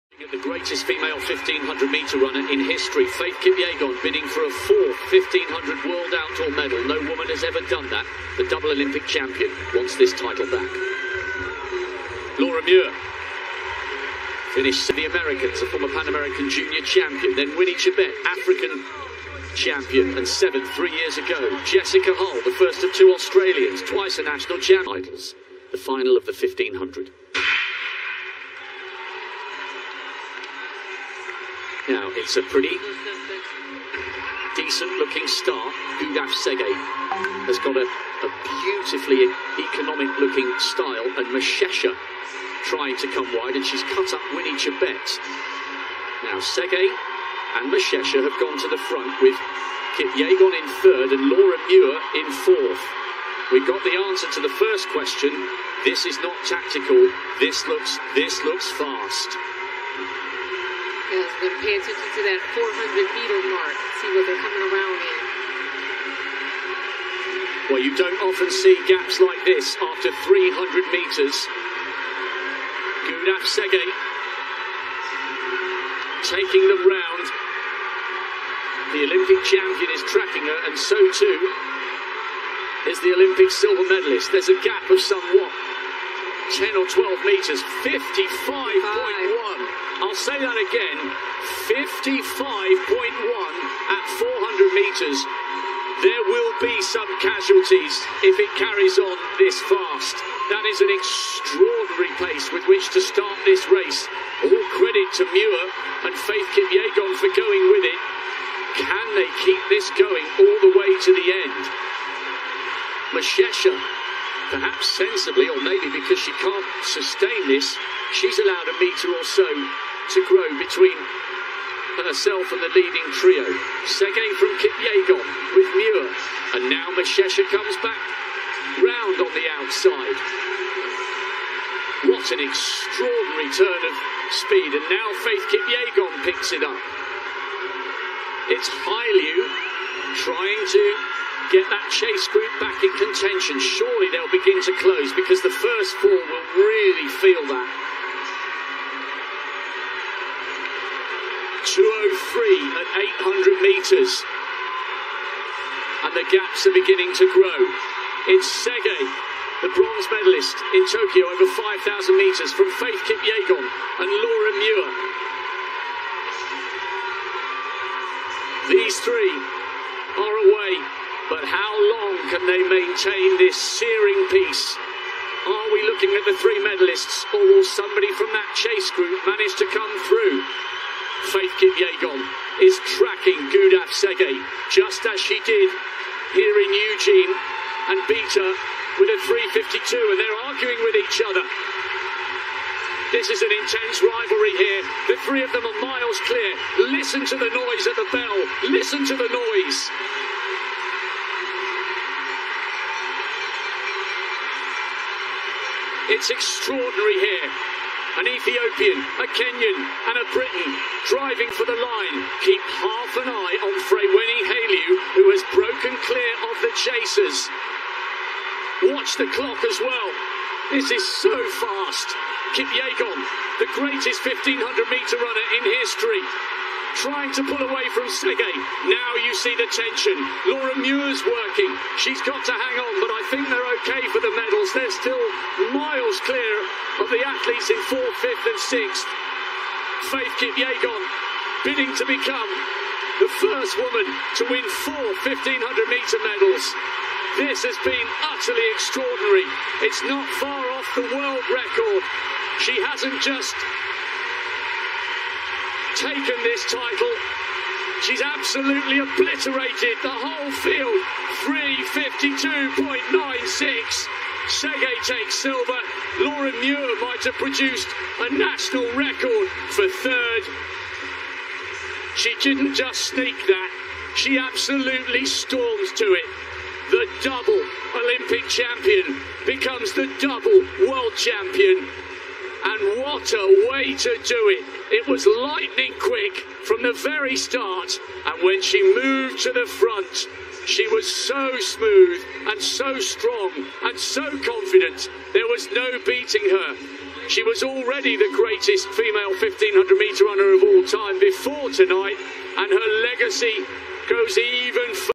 FAITH KIPYEGON won a Gold MEDAL 1500M Race.
Faith Kipyegon a Kenyan long race runner, has won the Gold medal in 1500m at Oregon USA.